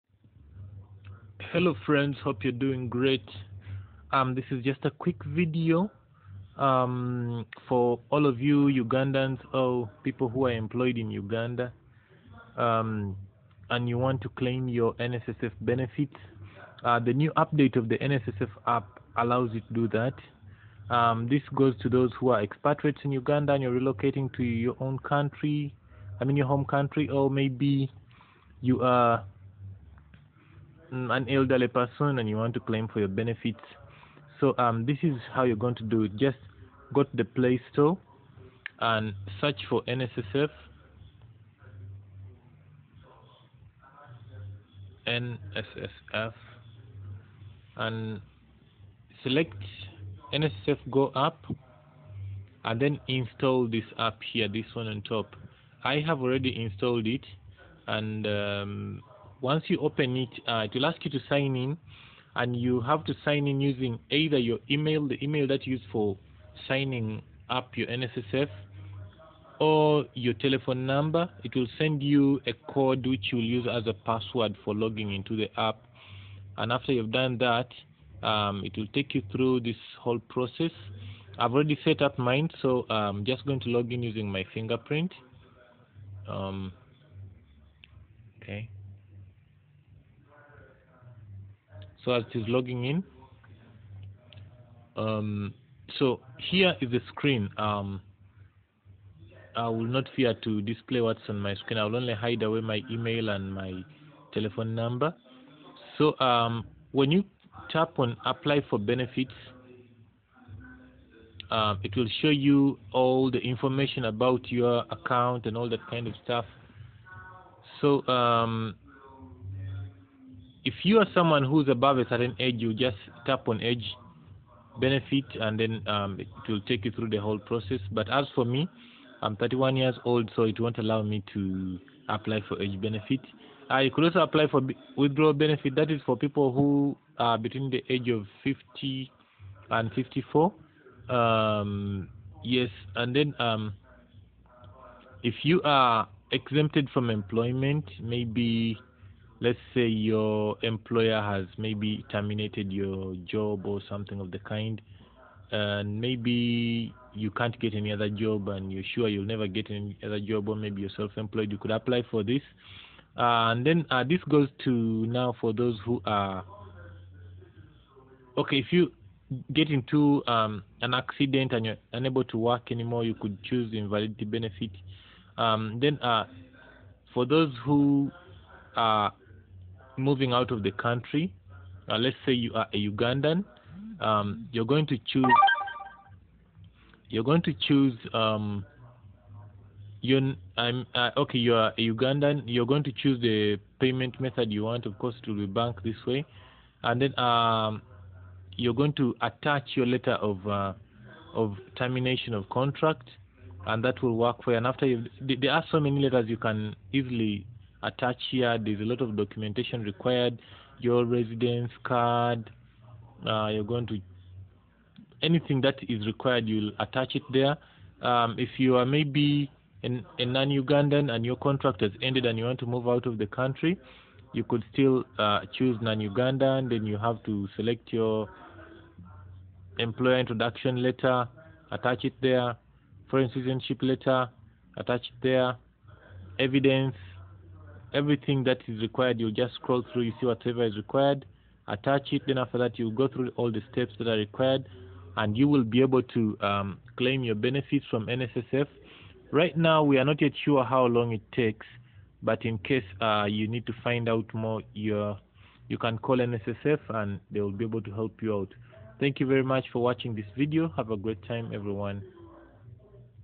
How to claim for your NSSF benefits in Uganda using your mobile phone (android app only)
In this video, I will show you how you can claim for your NSSF benefits as a Ugandan or an expatriate desiring to get out of Ugandan employment for good.